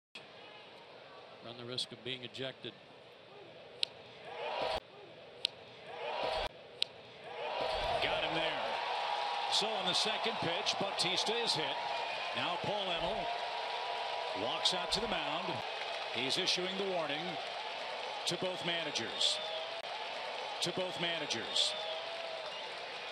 Julio Teheran hits Jose Bautista in the 1st inning
Julio Teheran hits Jose Bautista with a pitch during his first at-bat, forcing the home-plate umpire to warn both benches